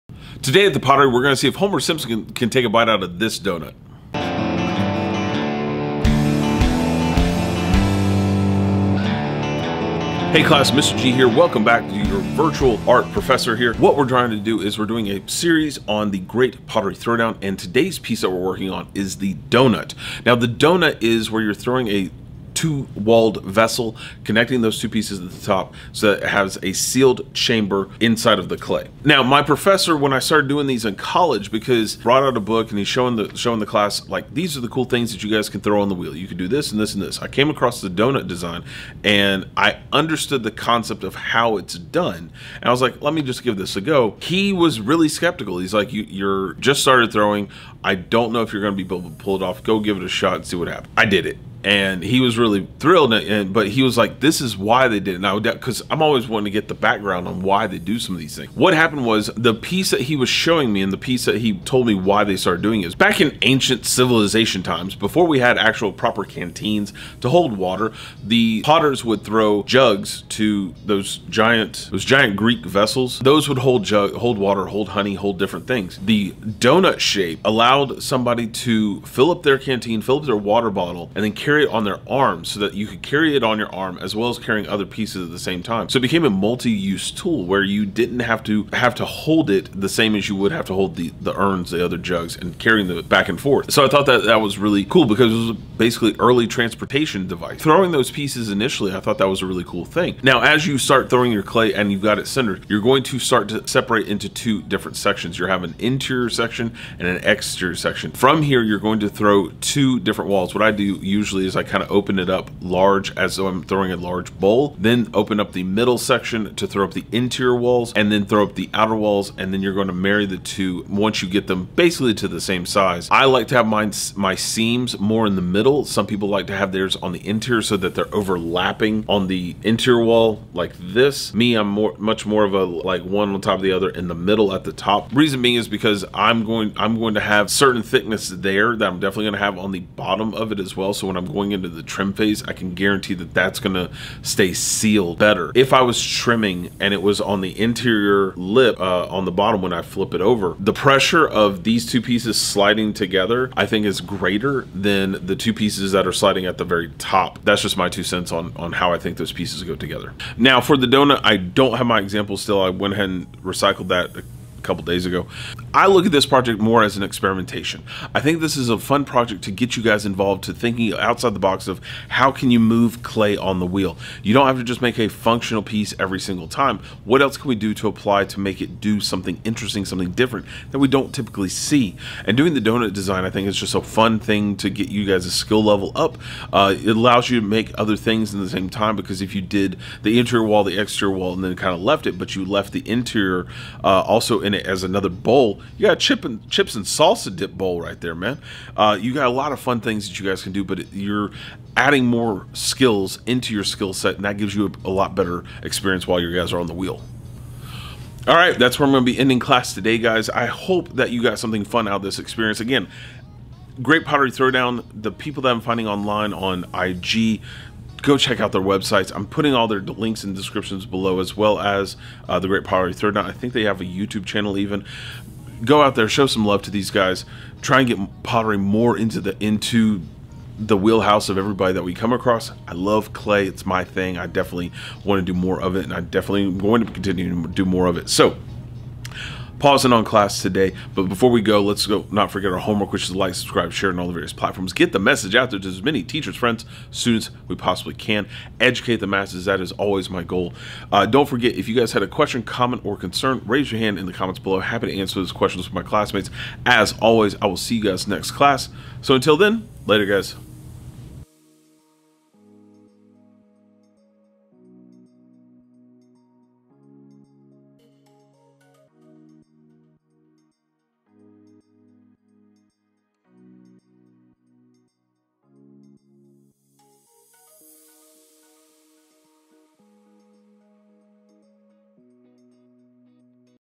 Clay Doughnuts to Start Your Day | Pottery Throwdown | University of YouTube
This week we are working on our summer ceramic series.
Are you a fan of The Great Pottery Throw Down?  I also enjoy using the pottery wheel and creating new pieces like clay rings, doughnuts, vessels, vases, and bowls in clay.
Learning new tips and tricks for the pottery wheel is a great way to improve your skills in ceramics and clay in general.
Working on throwing every day is the best way to increase your skills and build style in your portfolio.
Check out the following resources for more on this topic and leave a comment to help others with their clay knowledge or ideas.

Category
Education